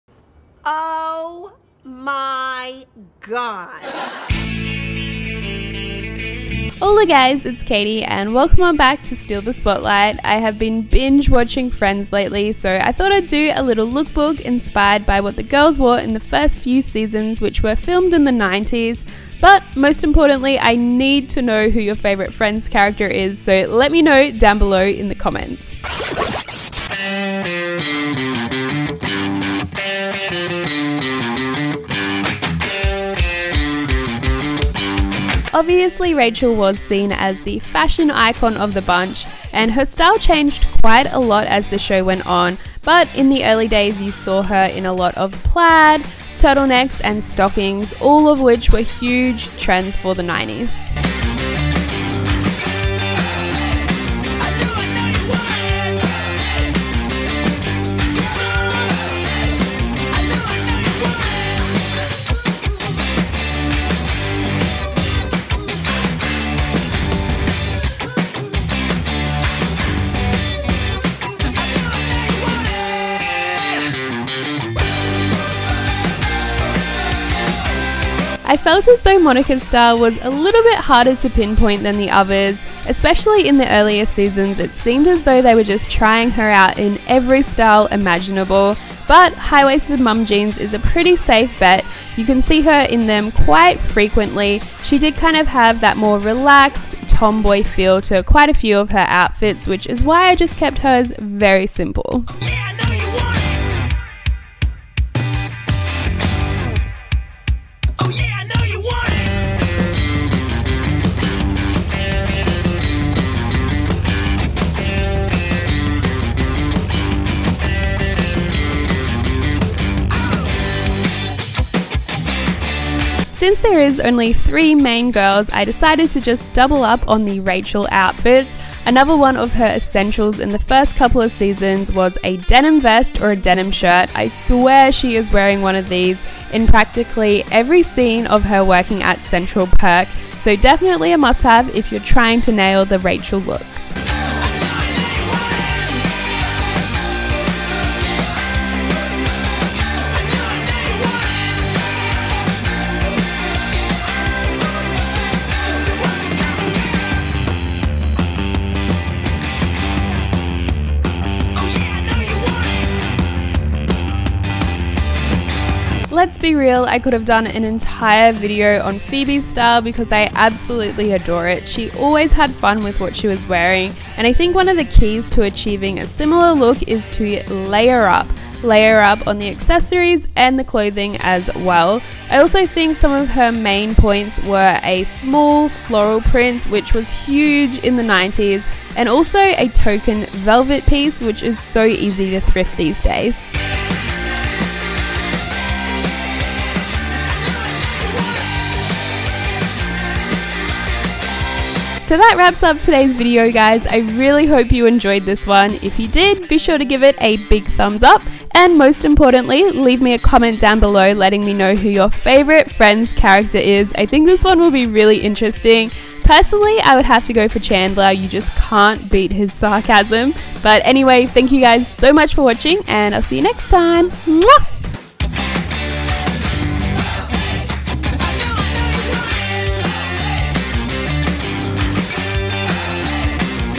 90's Outfits Inspired by Friends | Steal The Spotlight
90s Lookbook inspired by the outfits Rachel, Monica and Phoebe wore in the TV Show Friends! Hope you enjoy :)

Friends has always been one of my favourite shows and I had a few requests to do a lookbook inspired by what they wore so finally here it is!

REMEMBER TO LET ME KNOW WHO YOUR FAVE CHARACTER IS!

Shoes - Thrifted

Rachel's 2nd Look
Denim Vest - DIY (Used to be a Jacket)
Skirt - Supre (black not online)
Shoes - Thrifted